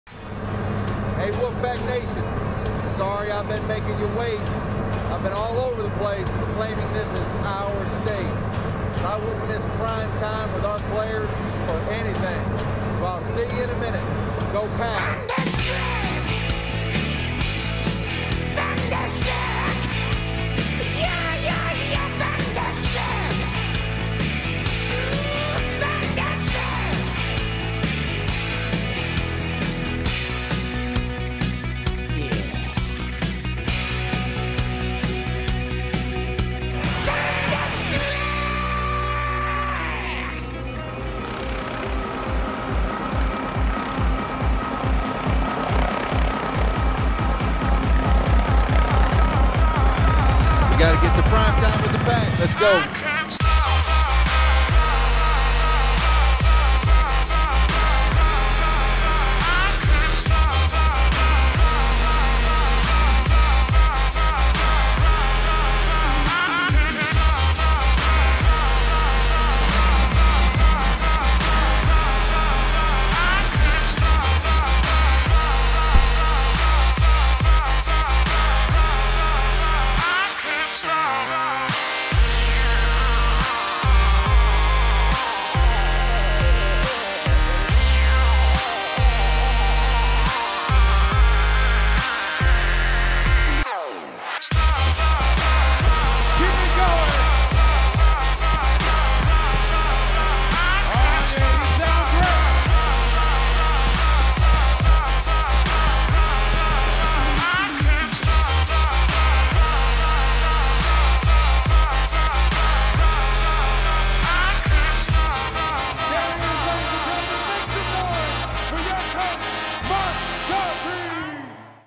NC State Coach Gottfried Primetime With Pack Entrance
Coach Mark Gottfried went to the extreme to make it to Primetime With the Pack, Operation Our State. Jumping out of a plane is just one part of his journey to the event.